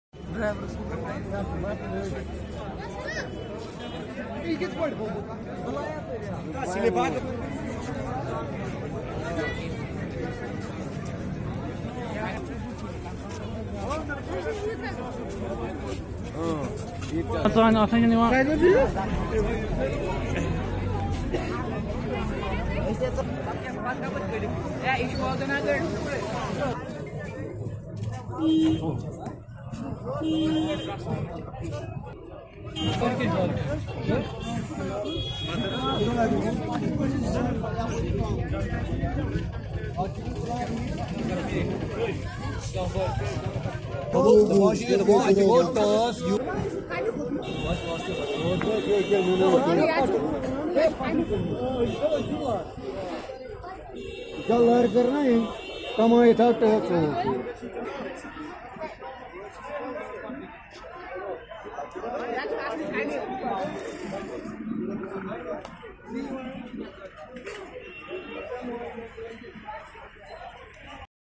Several injured after vehicle turns turtle in Srinagar’s Nawakadal
Srinagar: Several persons were injured after a vehicle they were travelling turned turtle in Srinagar’s Gratbal area on Monday.

Eye witnesses said that a Matador 407 vehicle overturned at Gratbal-Nawa kadal Nallah Mar road resulting in injuries to several persons.

The injured were quickly evacuated to Srinagar’s SMHS hospital for medical treatment.